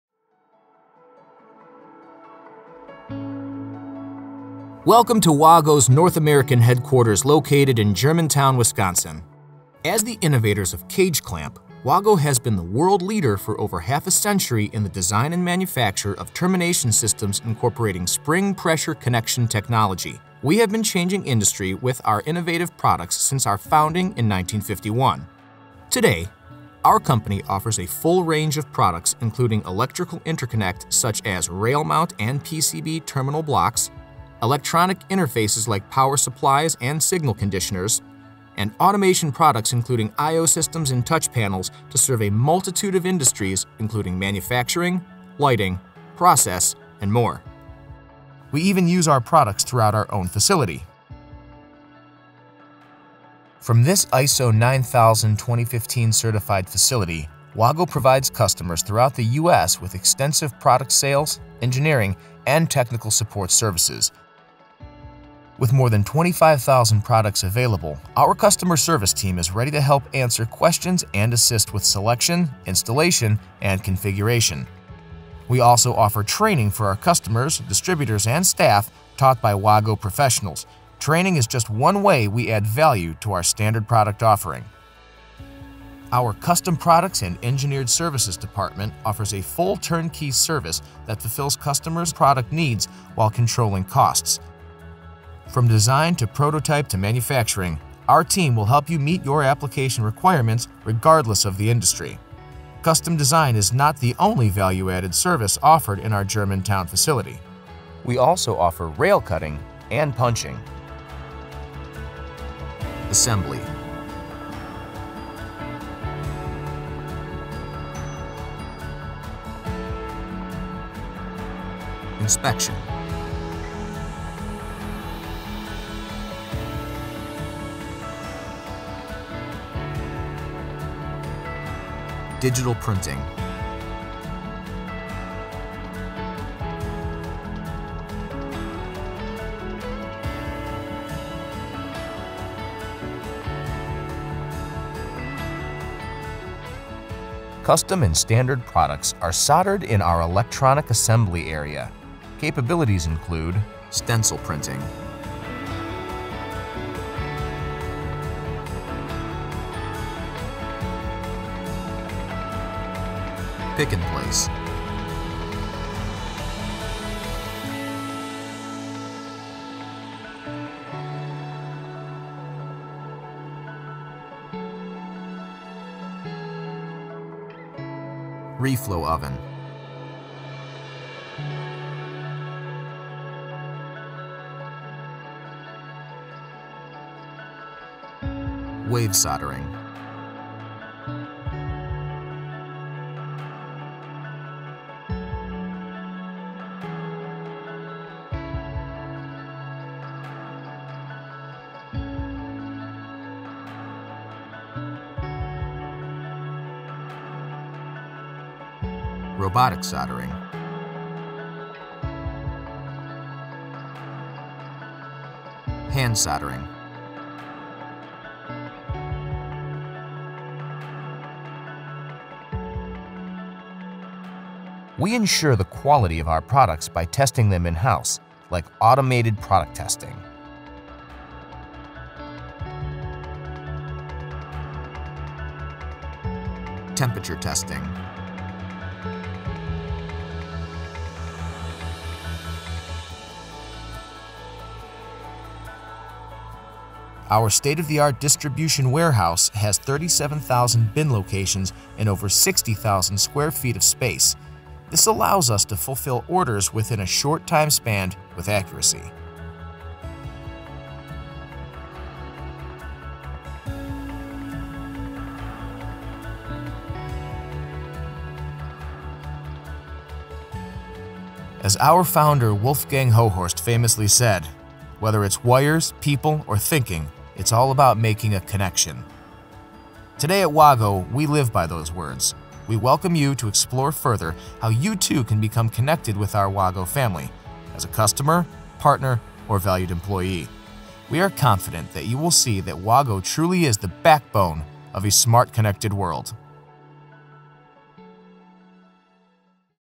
Welcome to WAGO
In this video, we see how WAGO’s North American headquarters in Germantown, WI helps the company continue its reputation as a world leader in the design and manufacture of termination systems incorporating spring pressure termination technology.

Today, the company offers a full range of products including rail mount and PCB terminal blocks, electronic interfaces, and automation products including I/O systems and touch panels to serve a multitude of industries including manufacturing, lighting, process, and more.